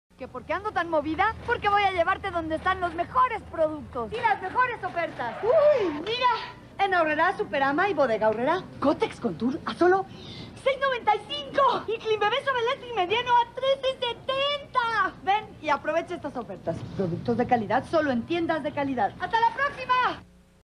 Comerciales mexicanos: Aurrera Superama Bodega Aurrera 1995
Con la participación de Maria Fernanda Garcia